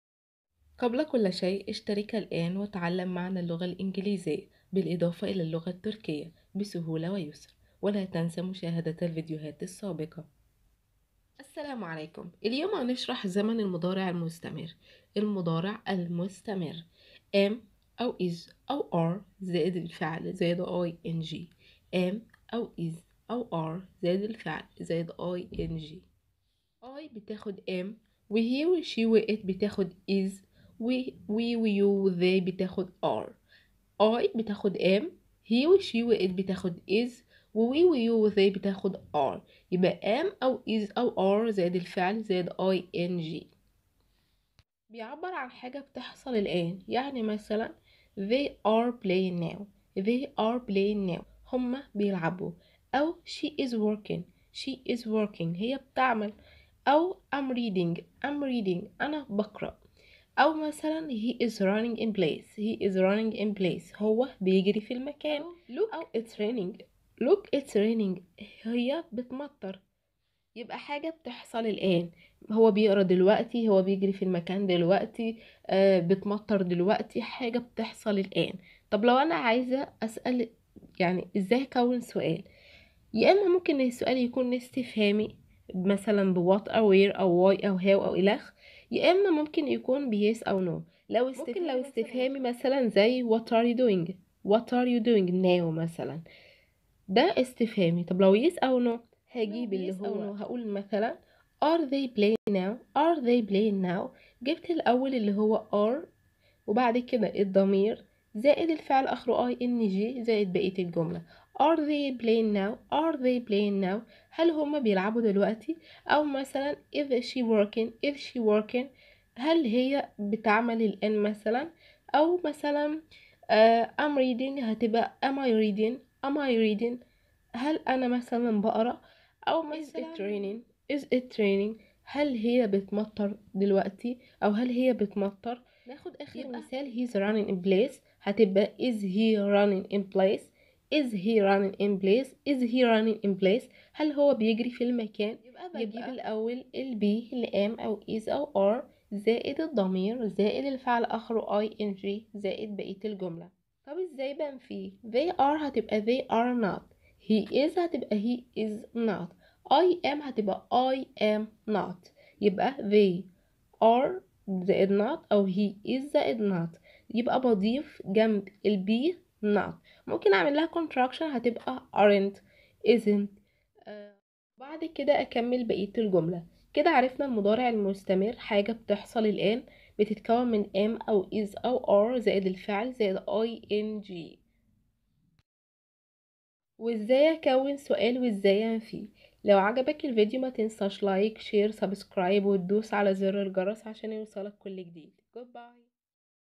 شرح زمن المضارع المستمر فى الانجليزية .The Present Continuous Tense
شرح زمن المضارع المستمر فى اللغة الانجليزية The Present Continuous Tense